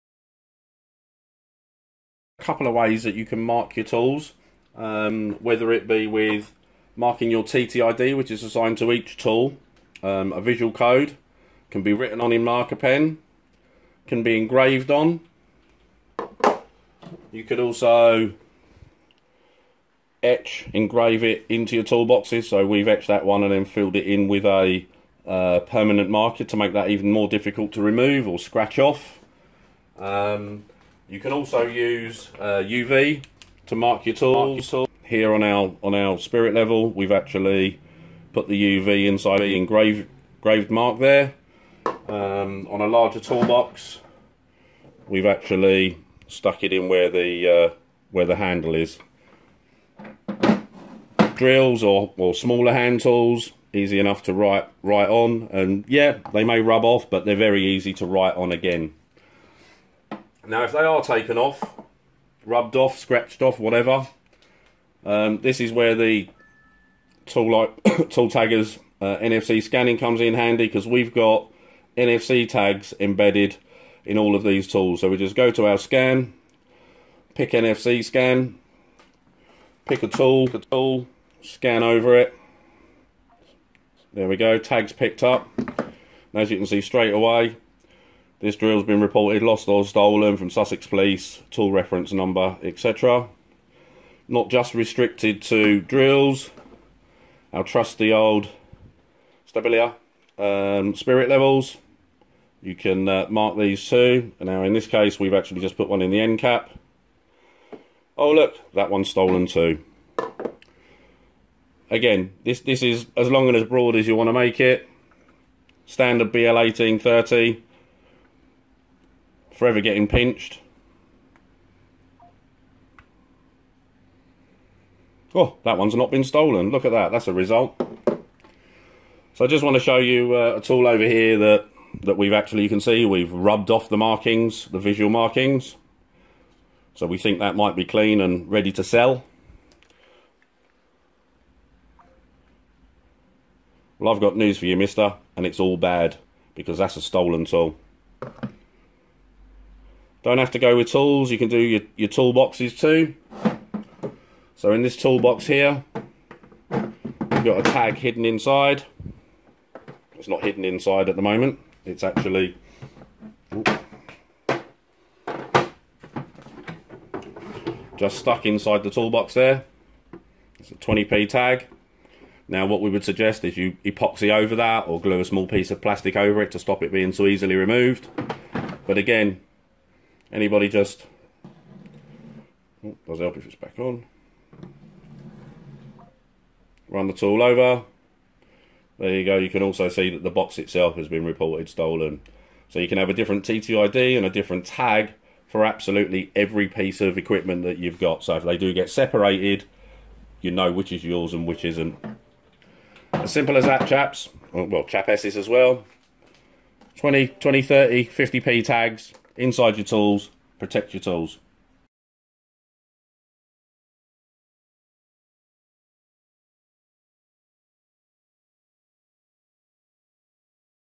Marking your tools with TTID's and NFC Tags
Colin @Tooltaggers demonstrates some of the ways to add your TTID's and NFC tags to your tools and toolboxes